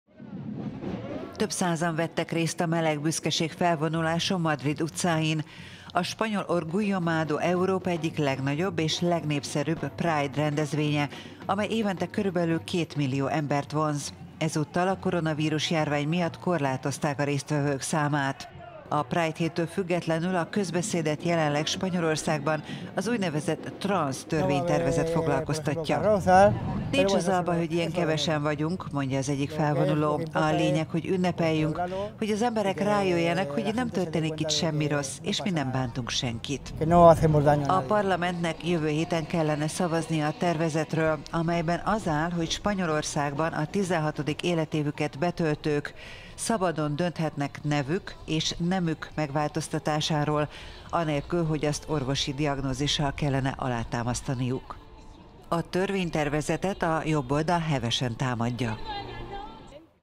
Pride Madrid a Covid árnyékában
Milliós tömeg helyett a járványügyi korlátozások miatt, néhány százan vehettek csak részt a spanyol meleg büszkeség meneten Madrid utcáin.